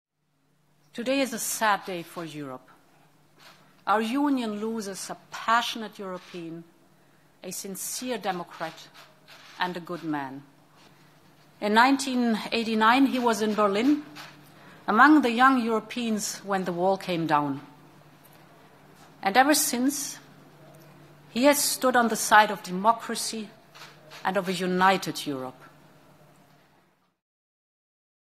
Von der Leyen zum Tod von Sassoli: "Trauriger Tag für Europa" | AFP
Der Tod von EU-Parlamentspräsident David Sassoli hat europaweit Bestürzung ausgelöst. EU-Kommissionspräsidentin Ursula von der Leyen sprach von einem "traurigen Tag für Europa". Die Europäische Union habe einen leidenschaftlichen Europäer und ernsthaften Demokraten verloren, sagte von der Leyen in Brüssel.